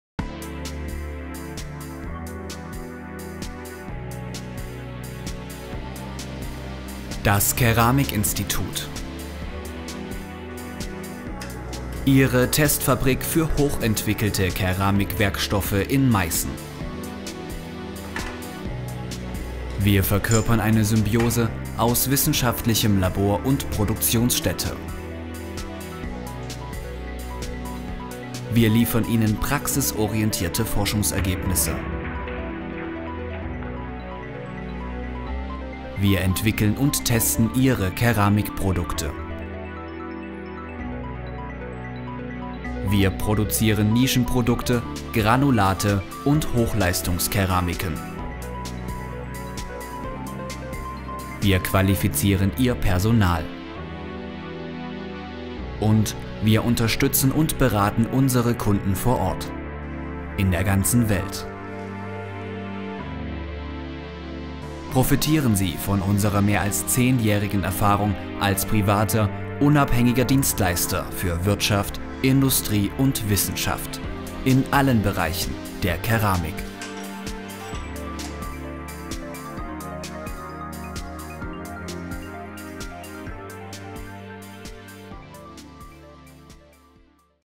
Imagefilm: Das Keramikinstitut Meißen
Imagefilm für "Das Keramikinstitut" in Meißen. Gedreht im Dezember 2010.